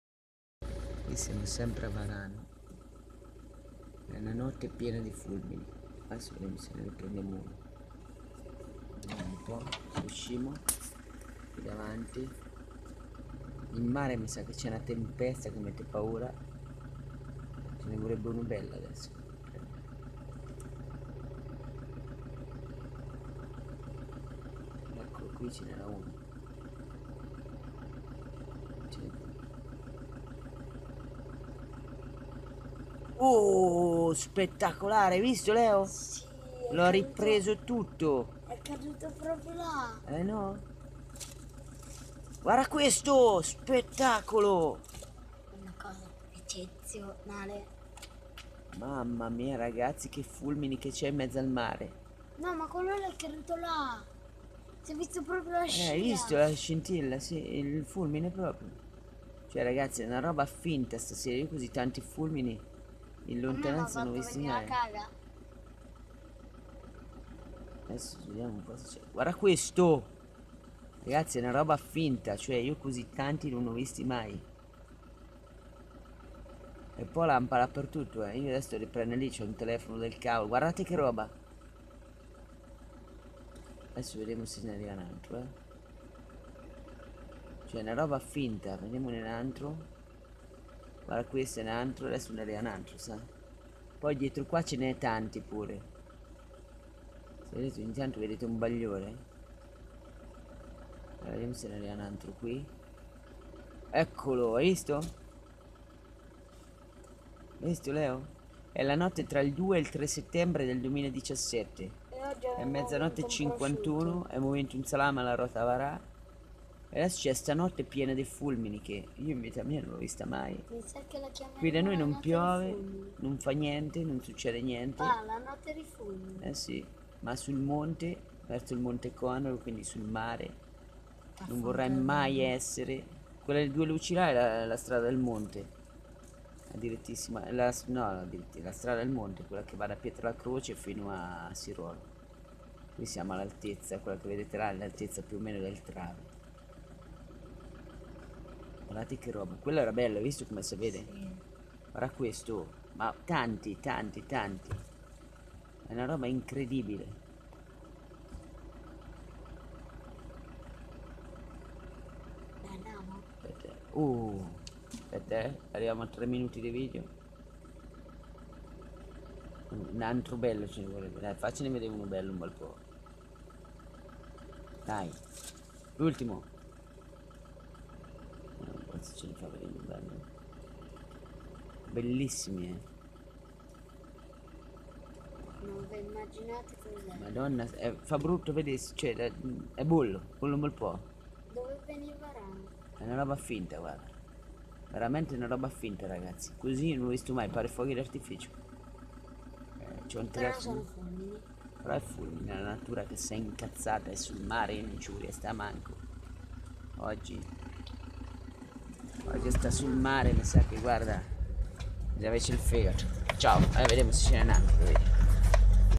Cacciatori di fulmini... Notte di maltempo su Ancona
BELLISSIMO SPETTACOLO...Nottata di fulmini sul capoluogo Dorico